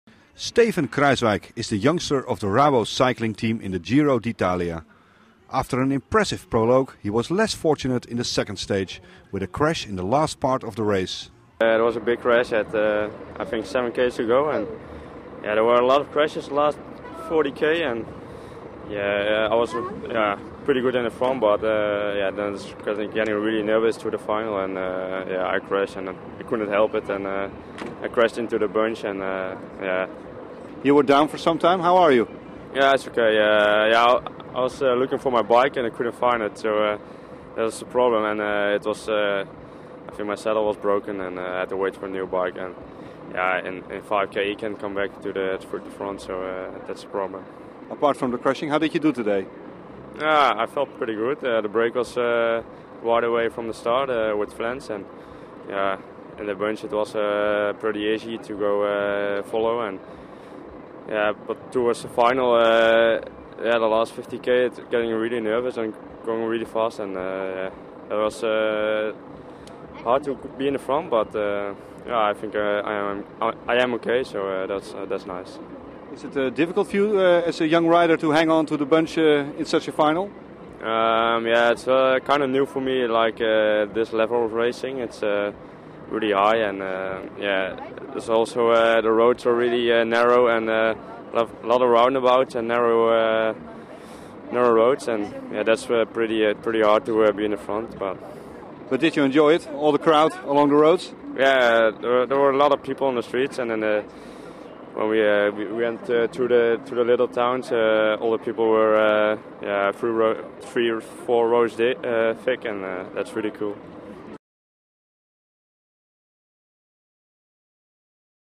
Kruijswijk enjoying Giro
Steven Kruijswijk, the youngster of the Rabobank Pro Cycling Team, enjoying the start of his first Giro despite todays crash. Kruijswijk attended the team at the last moment, replacing Oscar Freire.